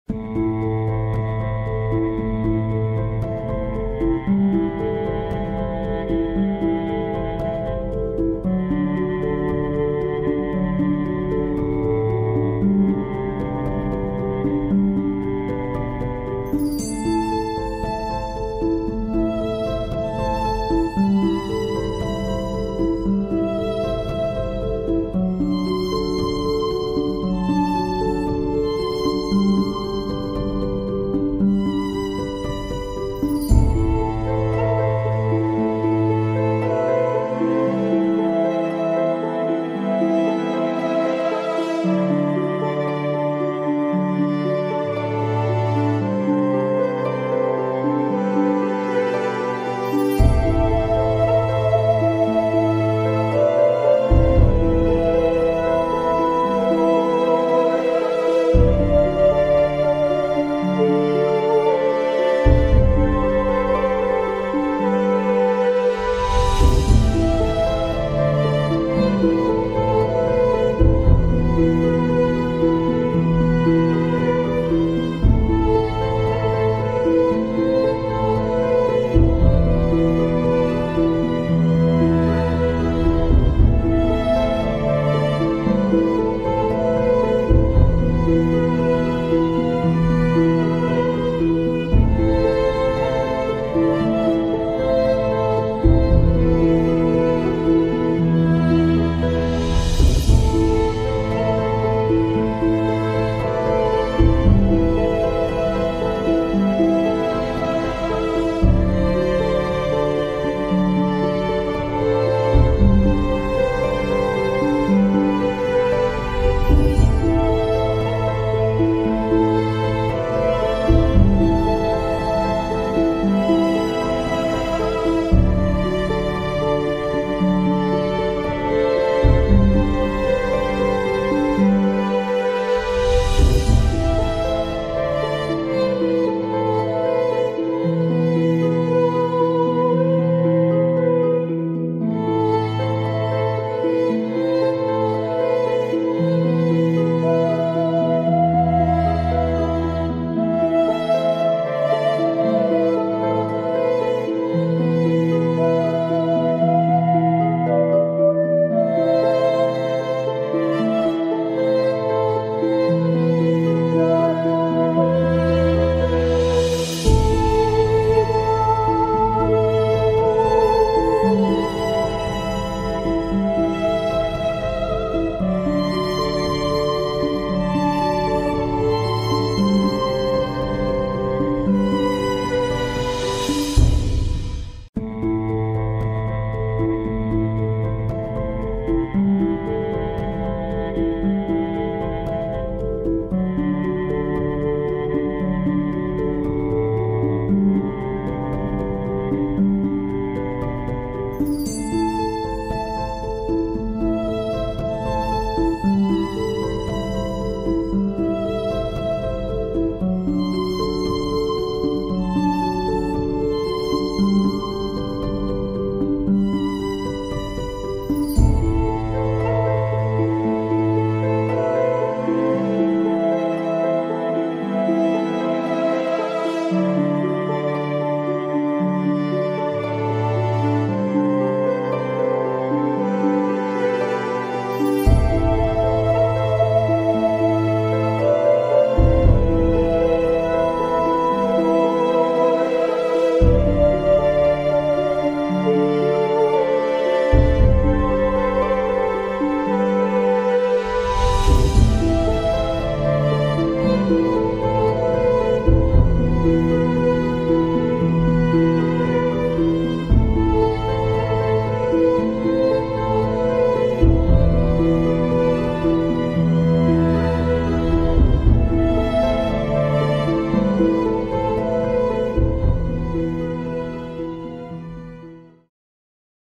Gene Key 50: Cosmic Order
Affirmations for the Gift of Equilibrium and Siddhi of Harmony using a 'Mind Movie'.